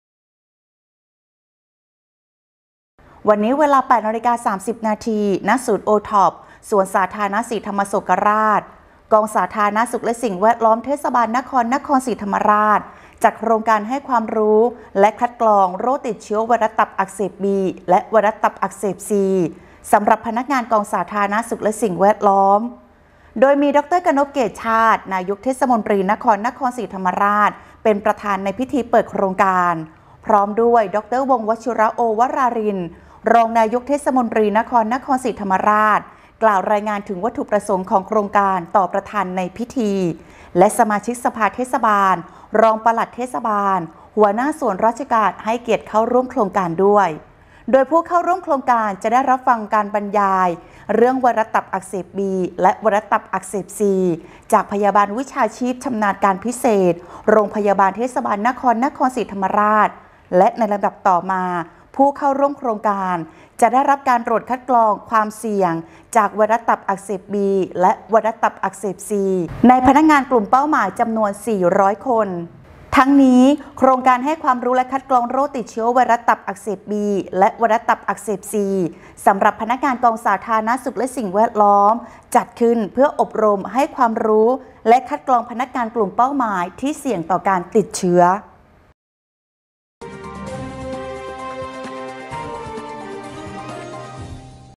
โครงการให้ความรู้และคัดกรองโรคติดเชื้อไวรัสตับอักเสบบีและไวรัสตับอักเสบซี สำหรับพนักงานกองสาธารณสุข
วันจันทร์ ที่  18 กรกฎาคม 2565 เวลา 09.00 น. ณ ศูนย์ OTOP สวนสาธารณะศรีธรรมาโศกราช  ดร.กณพ เกตุชาติ  นายกเทศมนตรีนครนครศรีธรรมราช เป็นประธาน เปิด “โครงการให้ความรู้และคัดกรองโรคติดเชื้อไวรัสตับอักเสบบีและไวรัสตับอักเสบซี สำหรับพนักงานกองสาธารณสุขและสิ่งแวดล้อม ”
โดยกองสาธารณสุขและสิ่งแวดล้อมมีพนักงานที่เกี่ยวข้องกับการรักษาความสะอาดและงานด้านกำจัดขยะมูลฝอยและสิ่งปฏิกูล รวมทั้งงานด้านอื่นๆ ที่เกี่ยวข้อง ซึ่งพนักงานกลุ่มนี้มีความเสี่ยงต่อผลกระทบทางสุขภาพและโอกาสเสี่ยงต่อการติดเชื้อไวรัสตับอักเสบบีและไวรัสตับอักเสบชี จากการถูกของมีคมทิ่มตำและจากการทำงานร่วมกัน อาจส่งผลต่อการแพร่เชื้อของโรคดังกล่าวได้
ทั้งนี้ กองสาธารณสุขและสิ่งแวดล้อมได้เล็งเห็นถึงความสำคัญของโรคดังกล่าวจึงได้จัดทำโครงการให้ความรู้และคัดกรองโรคติดเชื้อไวรัสตับอักเสบบีและไวรัสตับอักเสบชี สำหรับพนักงานกองสาธารณสุขและสิ่งแวดล้อมขึ้น โดยขอรับการสนับสนุนงบประมาณจากกองทุนหลักประกันสุขภาพเทศบาลนครนครศรีธรรมราช ประจำปีงบประมาณ พ.ศ.2565 มีวัตถุประสงค์เพื่อให้พนักงานมีความรู้ ความเข้าใจ และการป้องกันโรคไวรัสตับอักเสบบีและไวรัส ตับอักเสบชีและพนักงานกลุ่มเสี่ยงได้รับการตรวจคัดกรองโรค รวมถึงค้นหาผู้ติดเชื้อรายใหม่ให้ได้รับการรักษาหรือส่งต่อเพื่อการรักษาที่เหมาะสม  ซึ่งมีกิจกรรมที่สำคัญคือ การอบรมให้ความรู้ และการคัดกรองไวรัสตับอักเสบบีและไวรัสตับอักเสบซี ในพนักงานกลุ่มเป้าหมาย จำนวน 400 คน